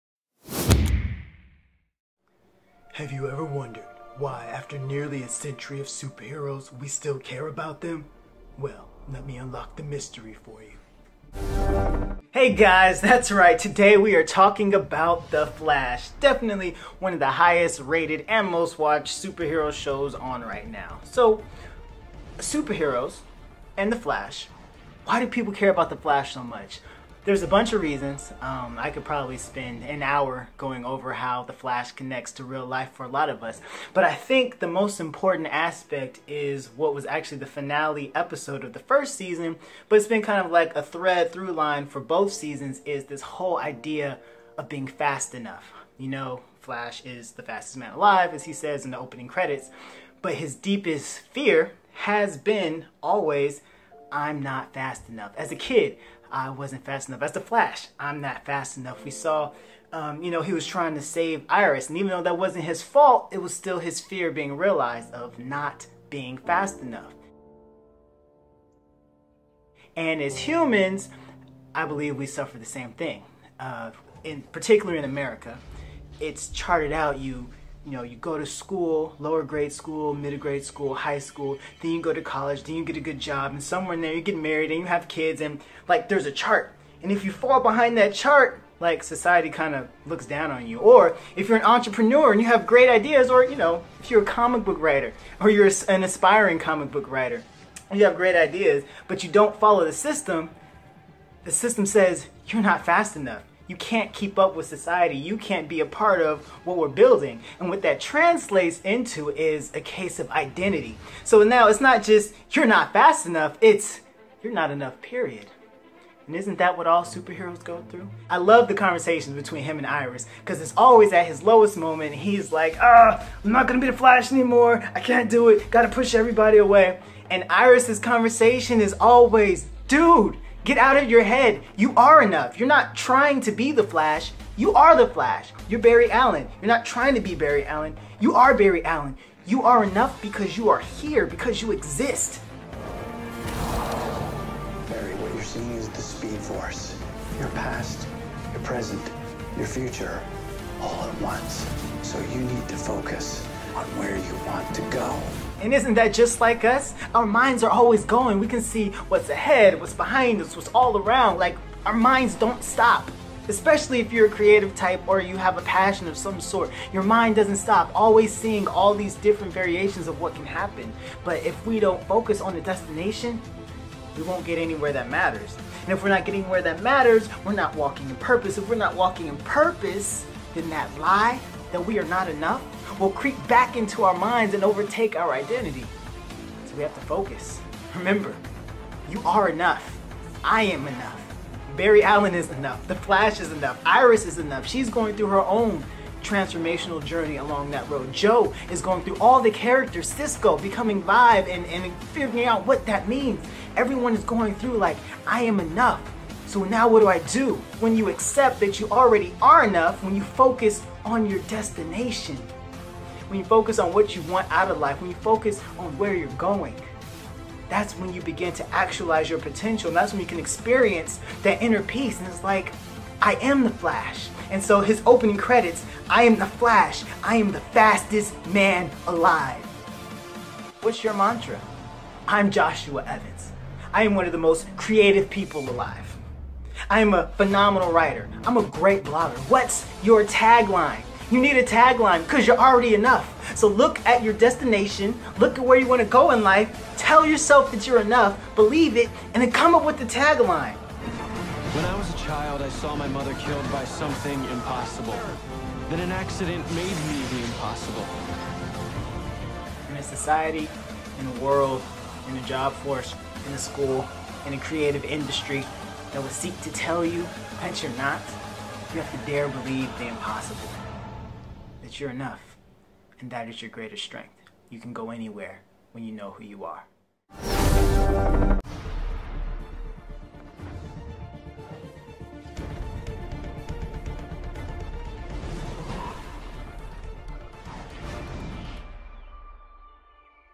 Fast Enough by Joshua Evans
Enthusiastically unlocking the connection between super heroes and real life! This is one reason, of many, why the Flash matters to us. Enjoy!